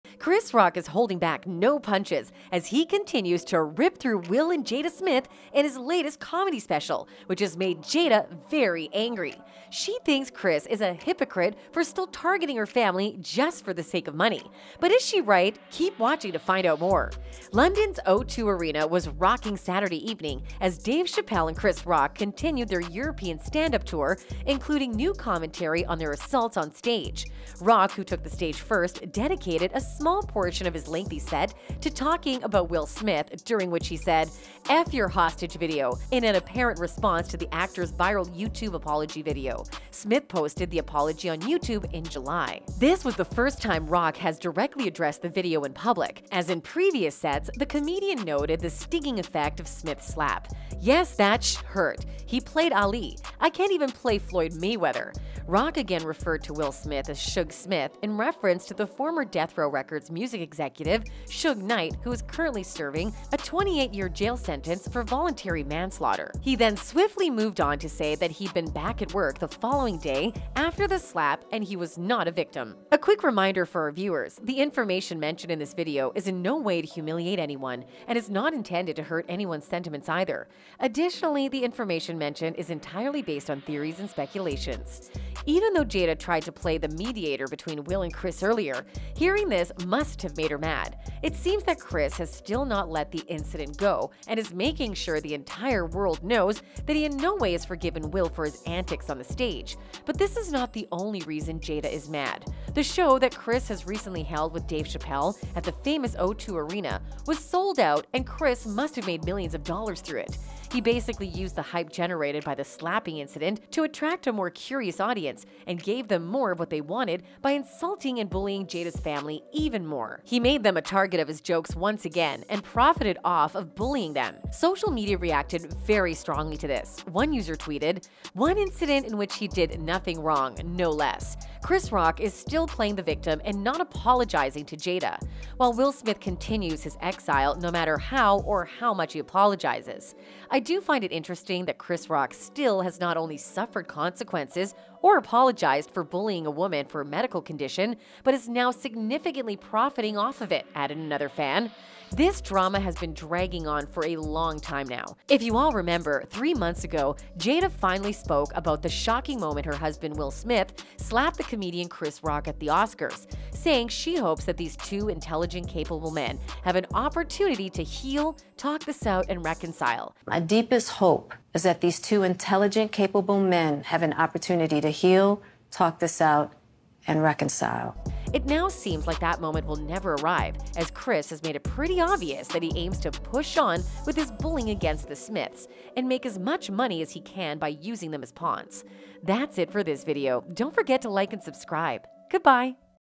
Jada Pinkett Calls Chris Rock HYPOCRITE After Still Exploiting Her Family For Money!
Jada Pinkett Calls Chris Rock HYPOCRITE After Still Bullying Her Family For Money!
Jada Pinkett has called Chris Rock a hypocrite after still bullying her family for money! Chris Rock is holding back no punches as he continues to rip through Will and Jada Smith in his latest comedy special, which has made Jada very angry.